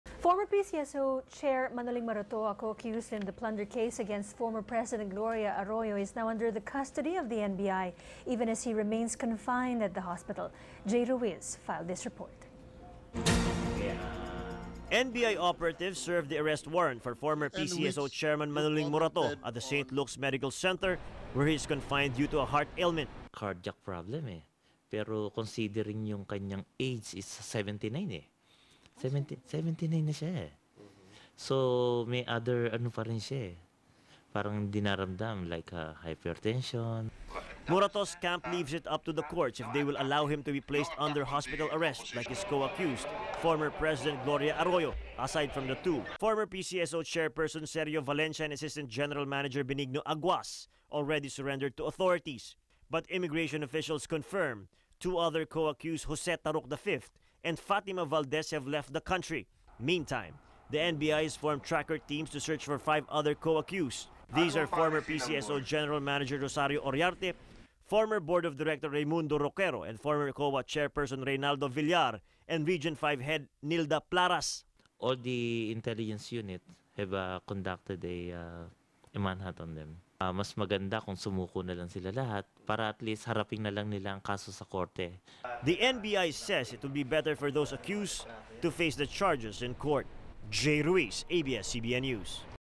Manoling Morato hospitalized, under NBI custody
Former PCSO Manoling Morato, a co-accused in the plunder case against former President Gloria Macapagal Arroyo is now under the custody of the NBI even as he remains confined in the hospital. Jay Ruiz filed this report.  Top Story, ANC, October 6, 2012